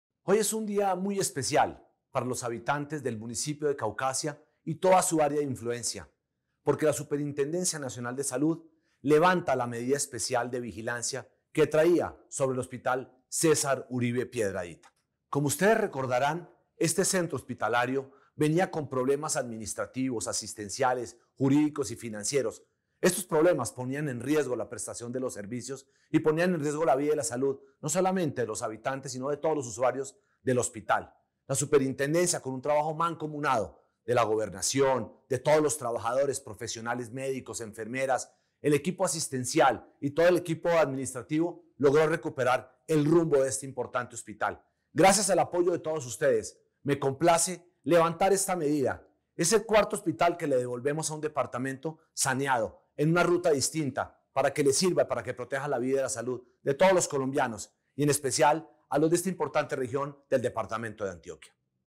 Supersalud levanta vigilancia especial al hospital César Uribe Piedrahita, de Caucasia (Antioquia)
Esta decisión de la Superintendencia se produce tras evidenciar que el centro hospitalario ha garantizado la adecuada prestación del servicio salud y ha trabajado para superar las fallas de carácter asistencial, administrativa y financiera que representaban un riesgo para los usuarios.

"Me siento complacido de ver cómo este hospital enderezó su rumbo y logró una recuperación asistencial y financiera que ya se siente en toda la red pública hospitalaria de la región”, aseguró el Superintendente Nacional de Salud, Fabio Aristizábal Ángel.